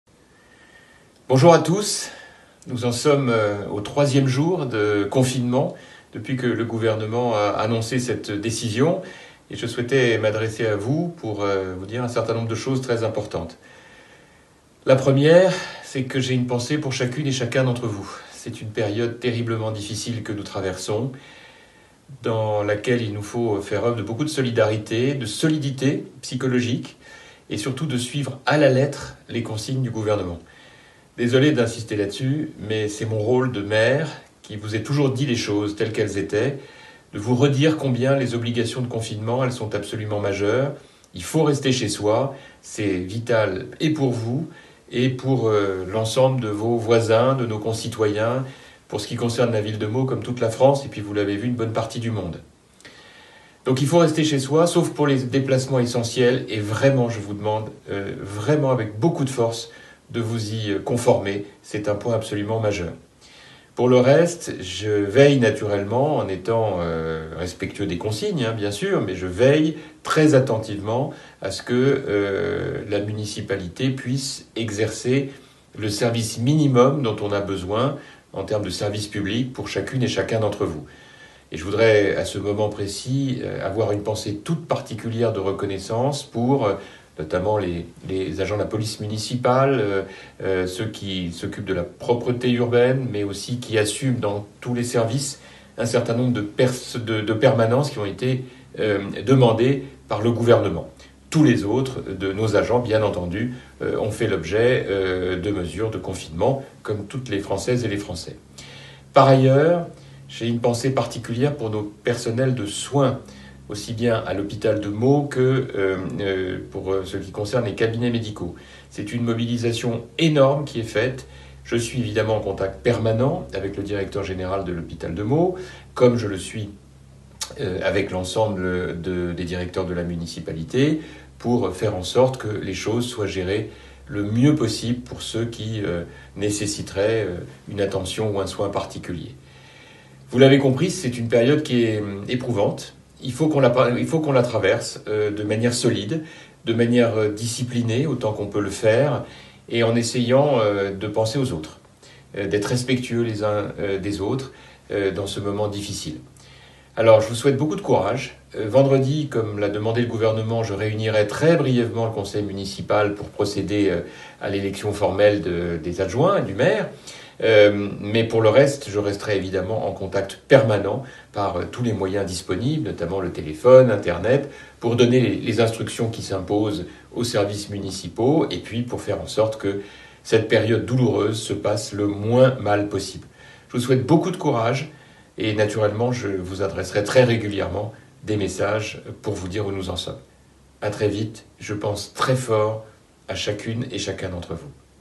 Confinement à Meaux : message de Jean-François COPÉ
Confinement à Meaux, message de Jean-François COPÉ : restez confinés au maximum !